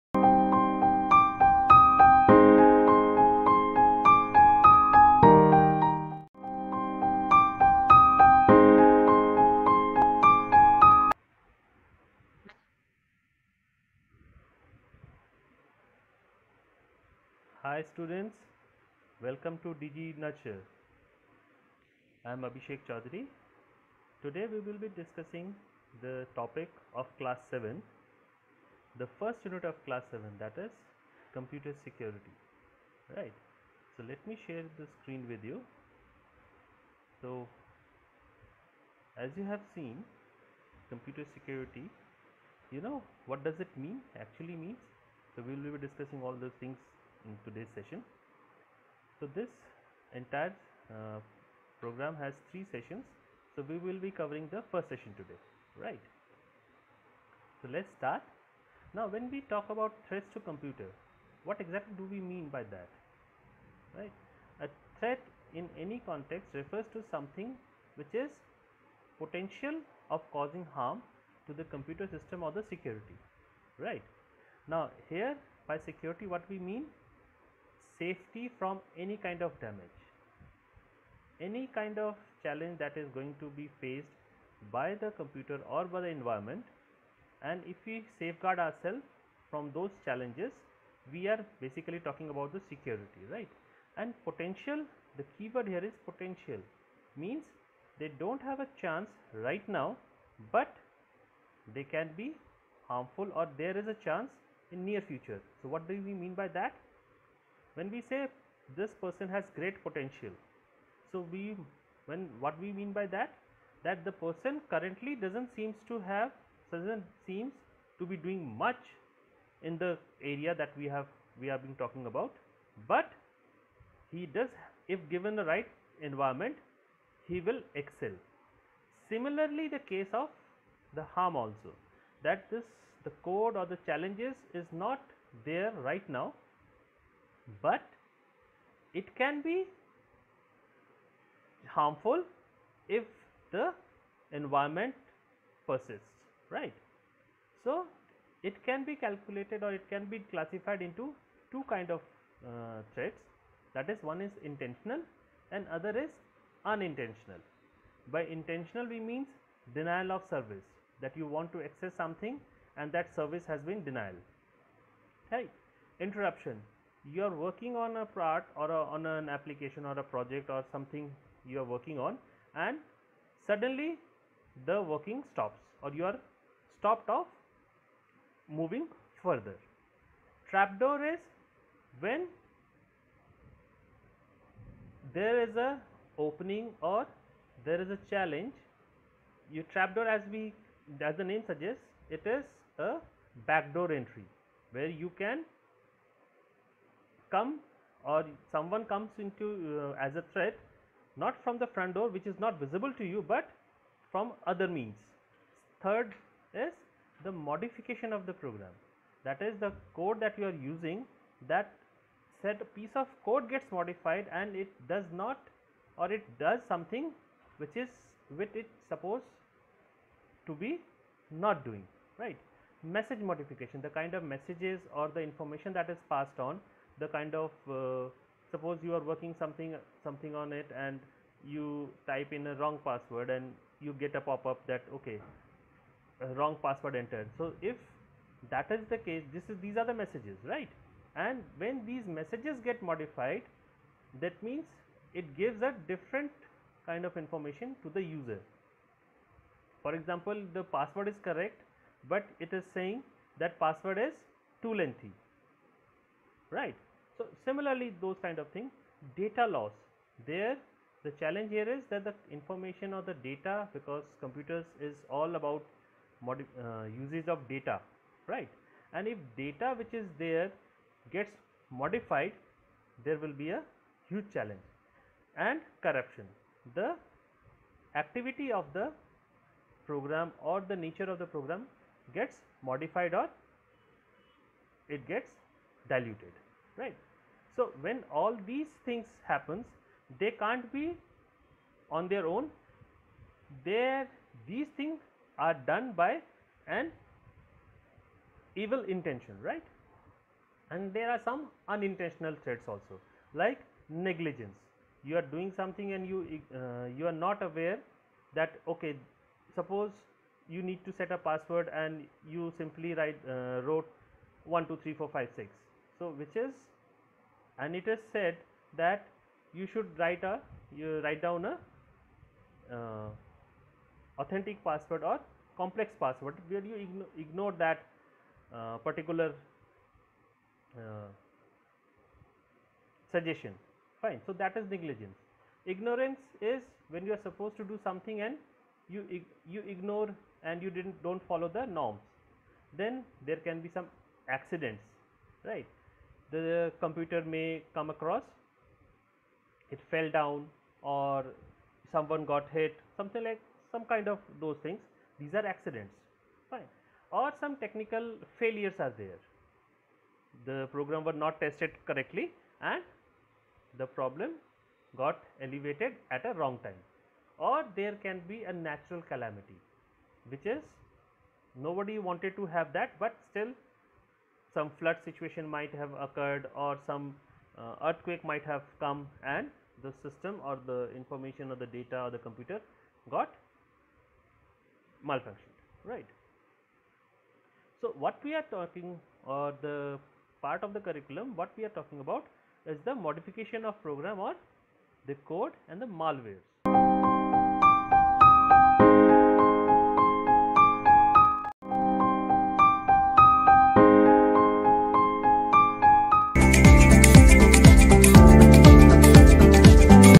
Computer Class 7 | Online Classes Demo | New Tutorial
Explore this demo of our Computer Class 7 online session, perfect for students who want to enhance their computer knowledge with engaging and easy-to-follow lessons. This video covers key concepts and practical examples to ensure students gain a comprehensive understanding. Whether you’re studying for exams or expanding your tech skills, this tutorial is an ideal resource for effective learning. Watch, learn, and improve your computer skills!